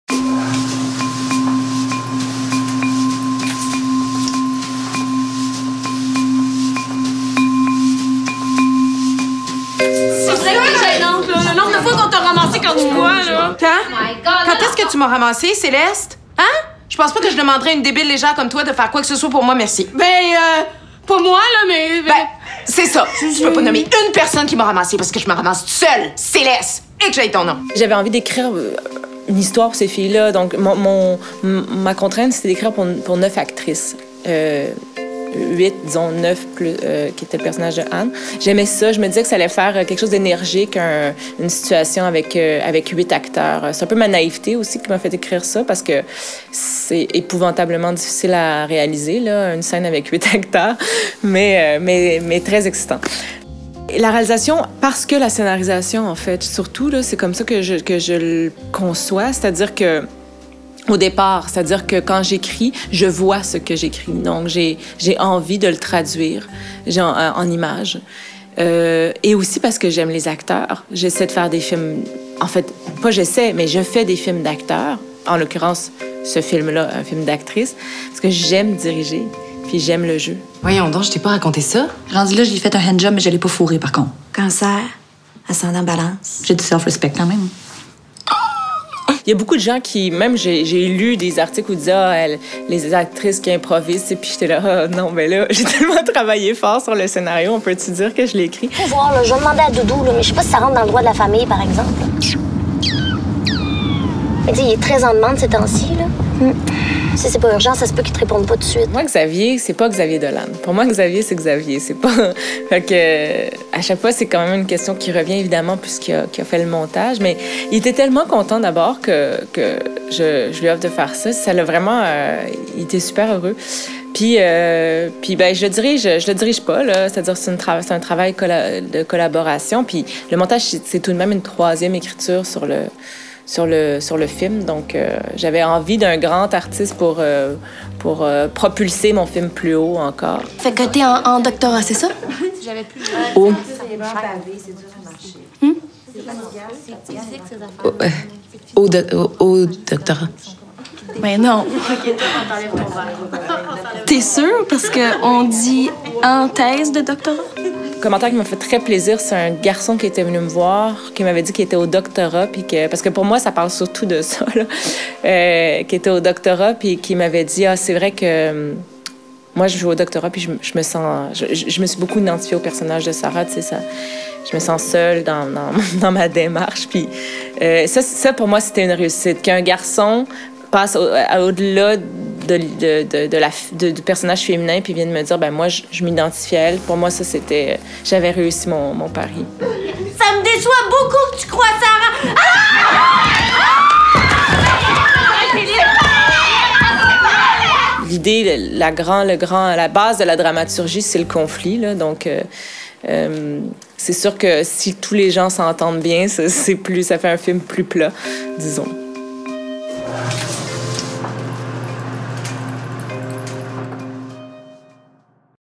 Interview with Monia Chokri - (english subtitles)
An interview with Monia Chokri, director of the short film An Extraordinary Person, presented Out of Competition at the 5th edition of MyFrenchFilmFestival.